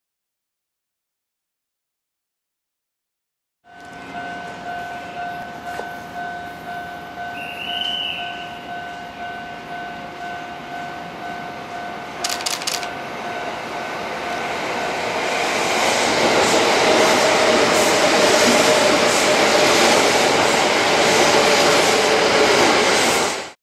【近鉄】9820系EH25編成(9825F)%快速急行三宮行＠鳴尾('13/06)
快速急行三宮行に充当される近鉄9820系9825Fを鳴尾で撮影した記録です。下り線が仮線ホームとなった後の記録です。
Namba-Line
Kintetsu Corporation
Naruo Station
Nishinomiya City Hyogo Pref. Japan
No.7768